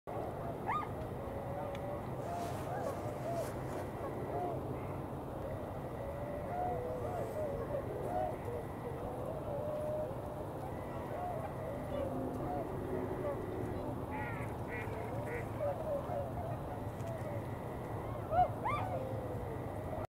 コガモがカーリングみたいに ぶつかって驚きます！【 Nussie Village / ぬっしー村 】
1月後半 8時頃 撮影